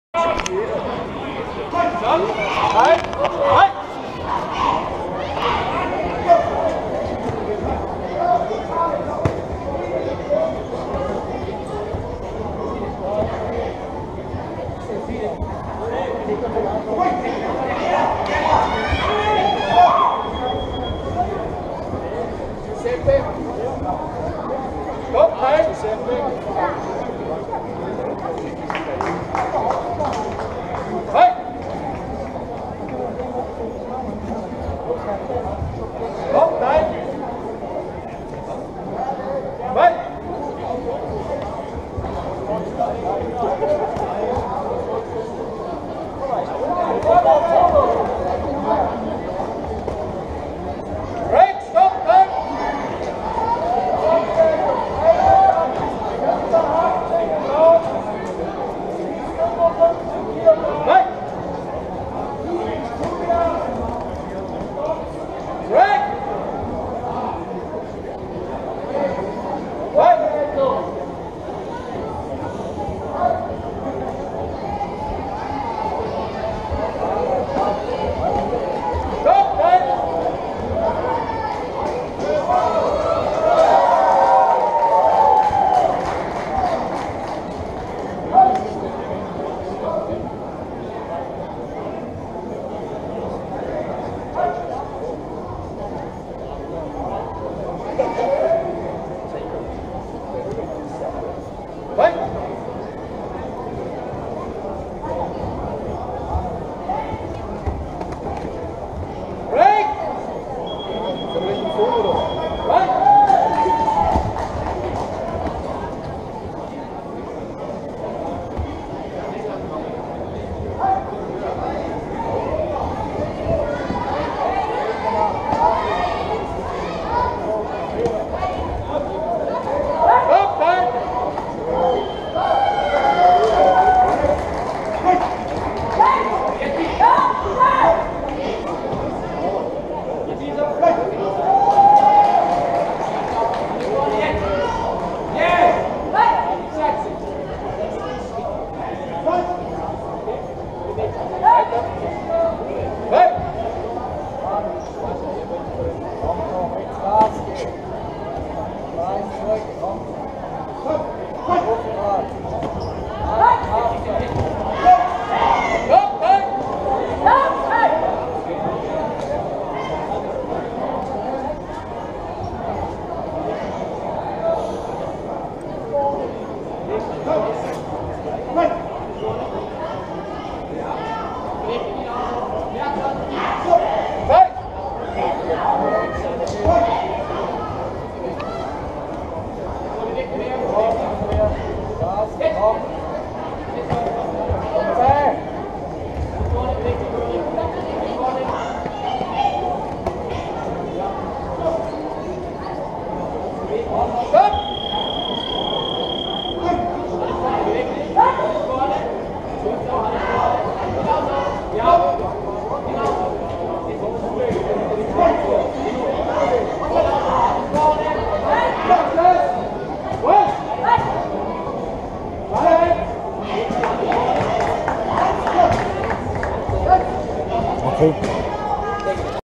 World Games 2017 WMAC - Arbon Schweiz - Kicklight - KARLS FIGHTCLUB Enrico Stoltz vs. ??
Vom 29. September bis 1. Oktober fanden in Arbon / Schweiz die Weltmeisterschaft - World Games – der WMAC und TAFISA (sportlicher Bereich der Vereinten Nation in New York) statt. Bei diesem Kampf in Kicklight war der eine Teilnehmer Enrico Stoltz  von KARLS FIGHTCLUB aus Freiburg und der andere Teilnehmer ist nicht bekannt.